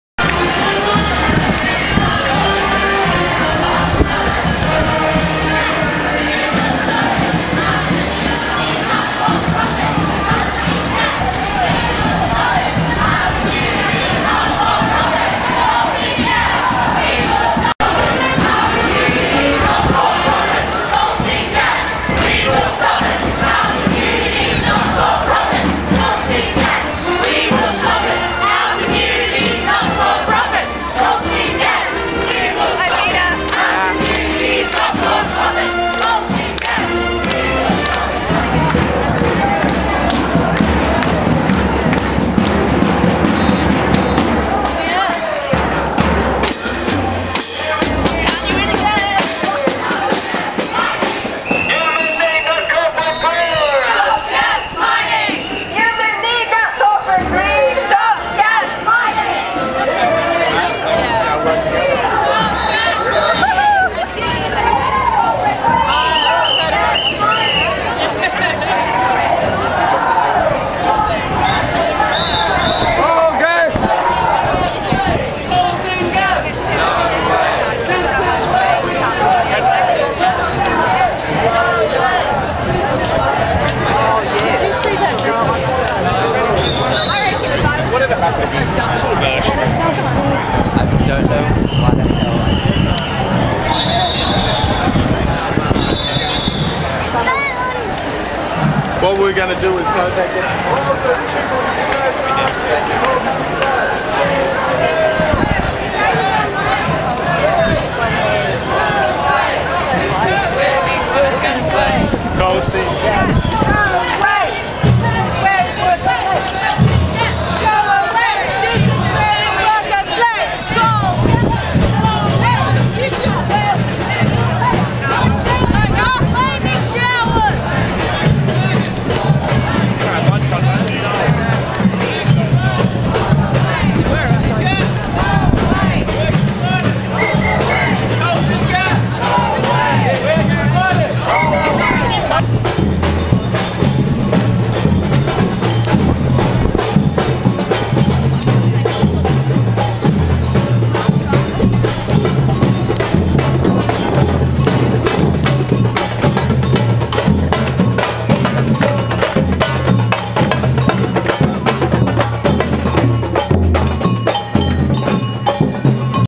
Sydney says: Stop CSG!
This is the start of a September 18, 2011 march against coal seam gas mining from the inner-city suburb of Newtown to St Peters, one of many Sydney suburbs where the NSW government has given mining companies to start drilling for coal seam gas.